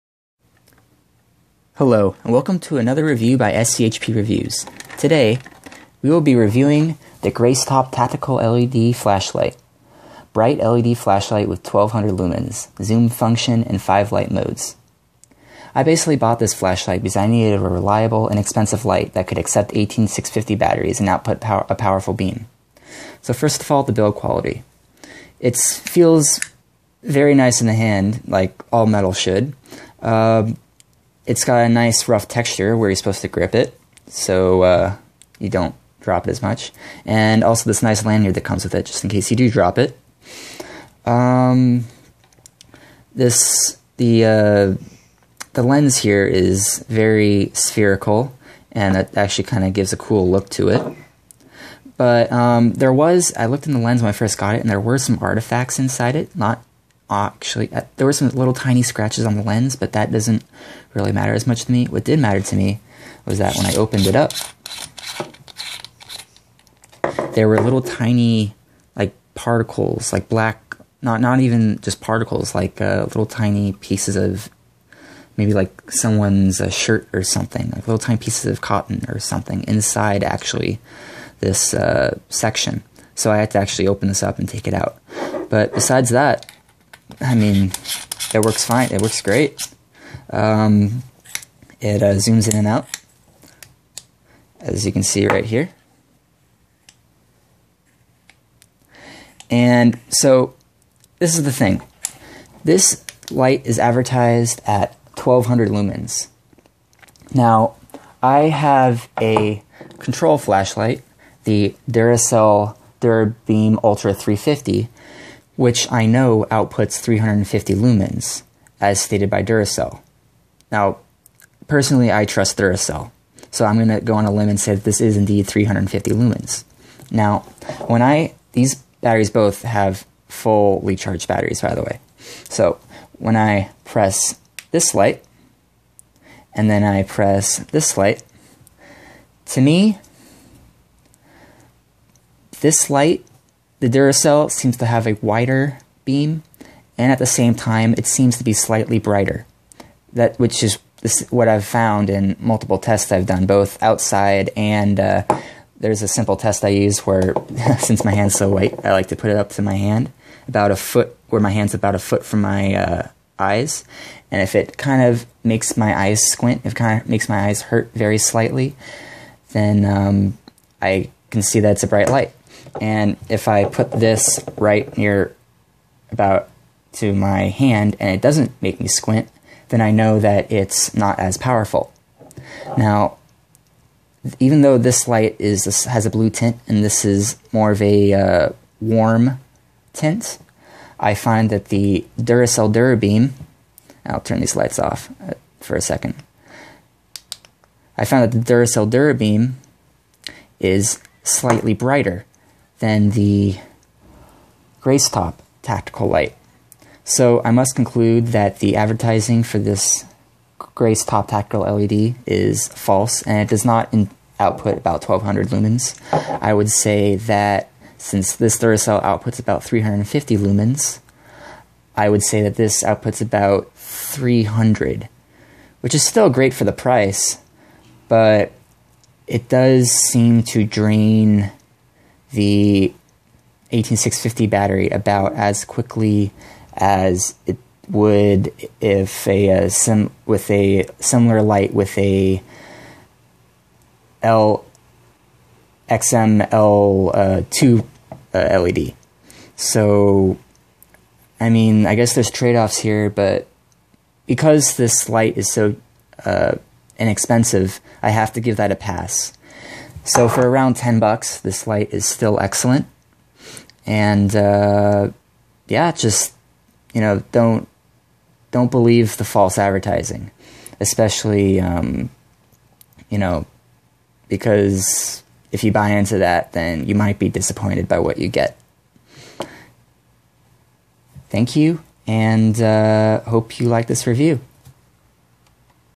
Gracetop Tactical LED Flashlight with 1200 Lumens, Zoom Function and 5 Light Modes Review
I bought the “Gracetop Tactical LED Flashlight Bright LED Flashlight with 1200 Lumens, Zoom Function and 5 Light Modes” because I needed a reliable, inexpensive light that could accept 18650 batteries and output a powerful beam.

The flashlight feels great in the hand.  It feels very solid, cold, and has a great amount of weight to it.  The top comes with sharp angled grooves so you can break something such as a glass panel if in an emergency, which is a great touch.  Where the flashlight is held, the metal has rough texture etched into it, so it is much harder to loose grip. If the flashlight is dropped by mistake, a lanyard also comes attached, so the flashlight is much harder to loose.

The lens is very spherical in shape.  This is interesting and actually gives a level of coolness to it.  Although there are particles and very small scratches, I was able to unscrew the lens and take out the particles.  The small scratches don’t make any noticeable difference in the spread or strength of the beam.
I then tested the battery life of this light.  With a 3000mAh 18650 battery, it lasted at near full power for about 2 hours, 30 minutes, which makes sense.

The disappointing aspect is that I then tested this light against the Duracell Durabeam Ultra 350, which outputs 350 lumens according to Duracell, a company I personally trust.  The maximum output is certainly not 1200 lumens.  At 1 inch from my hand, the Durabeam easily reflected off my hand and blinded me.  Then I tried this test with the Gracetop Tactical flashlight and it blinded me less so.  I continued to go back and forth with the flashlights, but with consistent results.

I must conclude that the “Gracetop Tactical LED Flashlight Bright LED Flashlight with 1200 Lumens, Zoom Function and 5 Light Modes” outputs less than 350 lumens.  I would guess around 300 lumens.

Still, this is a great flashlight.  Regardless, I know of similar merchants that offer near the same light with the same LED, but advertise a much more accurate lumen output.  For that I must knock off one star.

I give this product 4-stars.

** Every test and comparison was started exclusively with fully charged batteries.